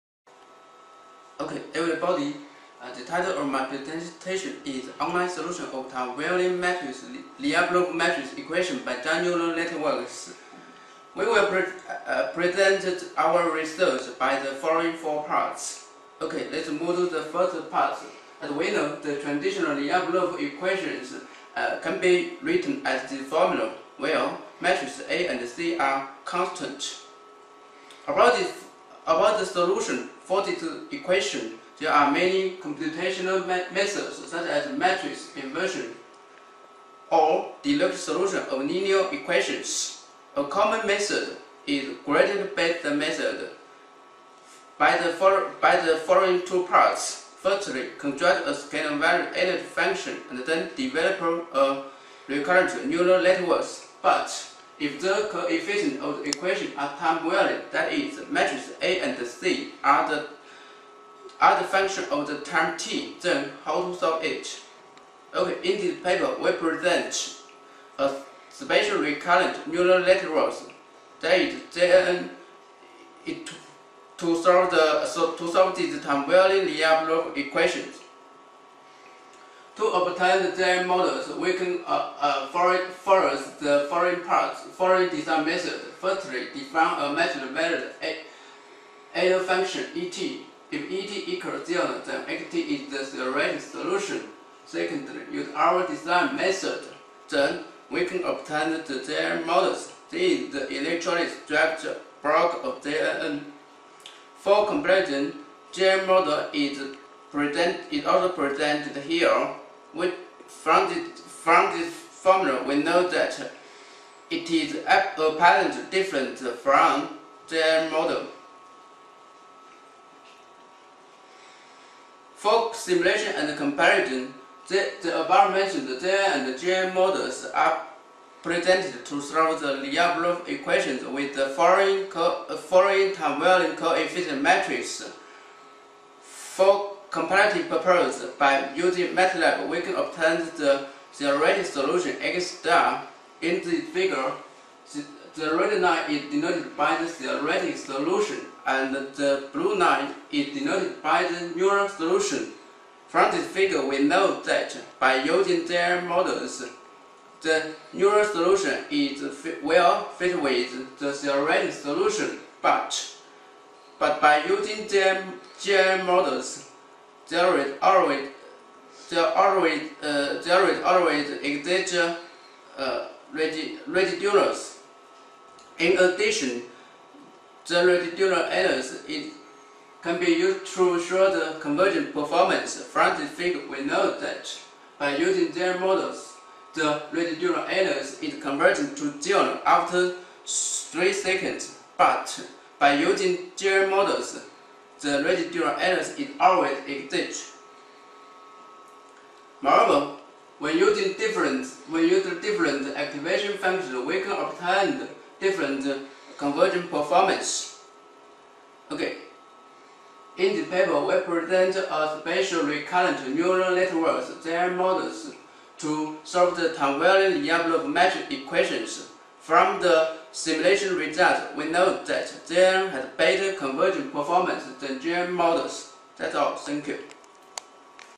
Online Solution of Time Varying Lyapunov Matrix Equation by Zhang Neural Networks
Title: Online Solution of Time-Varying Lyapunov Matrix Equation by Zhang Neural Networks
Journal: Recent Patents on Computer Science
Authors: Chenfu Yi & Yuhua Liu
Issue number: 6, 3(2013)

Abstract: By constructing a matrix-valued unbounded error-function, this paper develops and exploits a new type of recurrent neural networks, named as Zhang neural networks, for the time-varying Lyapunov matrix equation with accuracy and effectiveness. In general, a scalar-valued norm-based energy function is defined for the design and development of the conventional gradient-based neural networks, which could only solve the time-invariant matrix equation exactly. Comparison with some recent patents on the neural networks designed originally for the time-invariant problems solving, the patents relevant to Zhang neural networks is designed for the solution of time-varying problems based on the matrix/ vector-valued error function. An illustrative example substantiates that the presented Zhang neural networks can effectively solve such matrix equation with time-varying coefficients, while the conventional gradient-based neural networks could only approximately approach to the theoretical solution.